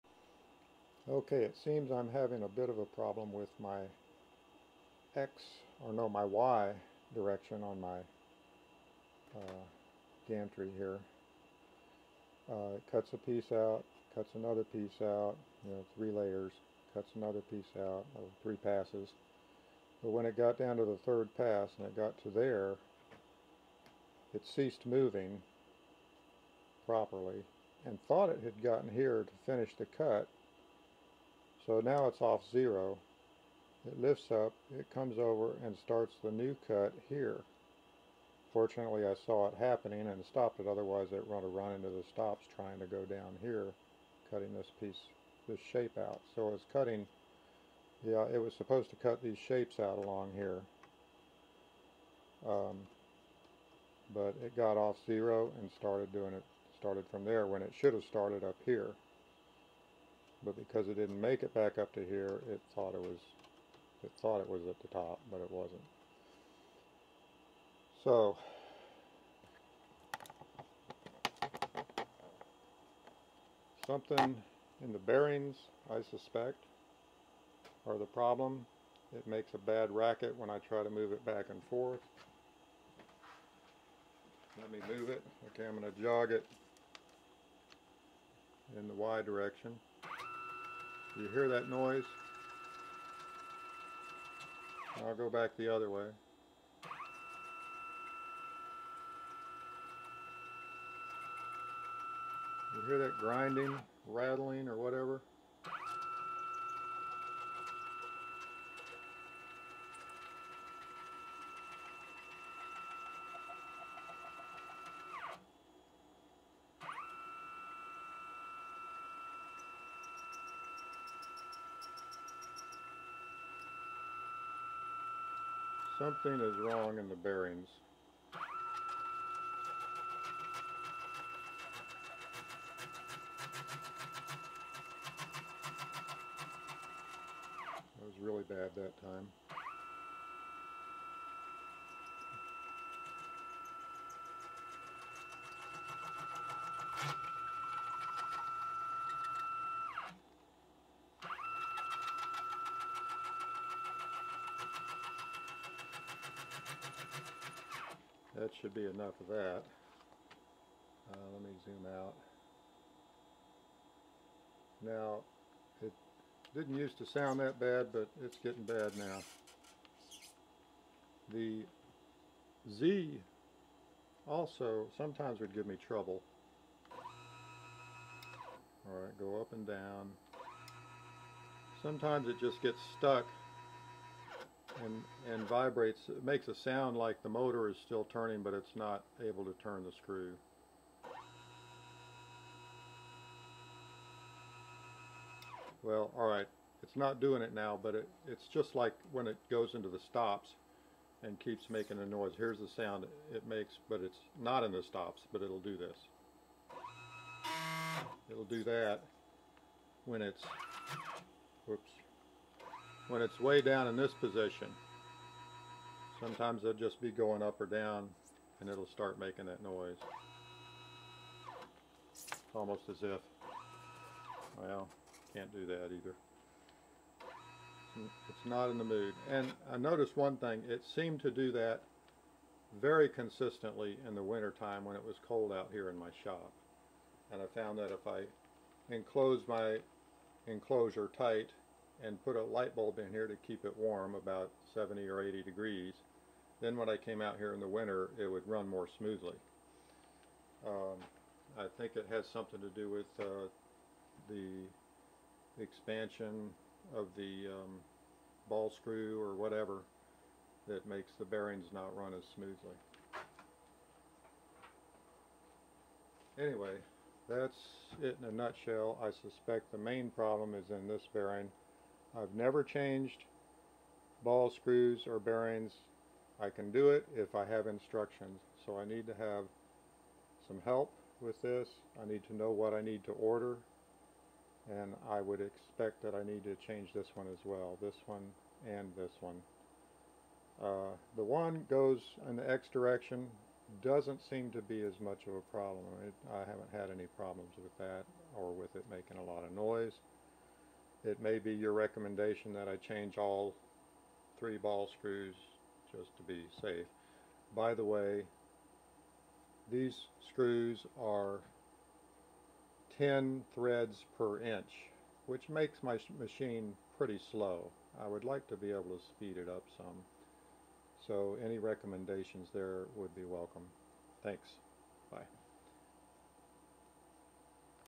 cncproblems
Description of problems with my K2CNC machine. I think the ball screw bearings are bad. Hopefully you will be able to identify the sound which seems to be coming from the ball screw bearing.  Also, the ball screws are 10 TPI which makes my machine rather slow. I'm open to recommendations to speed it up.  BTW, I bought the machine used, from someone who put it together himself.